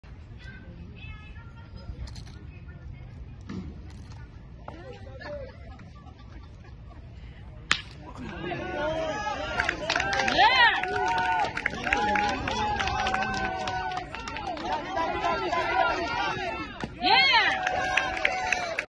Joaquin Huerta / 2023, SS, IL
Single into left center gap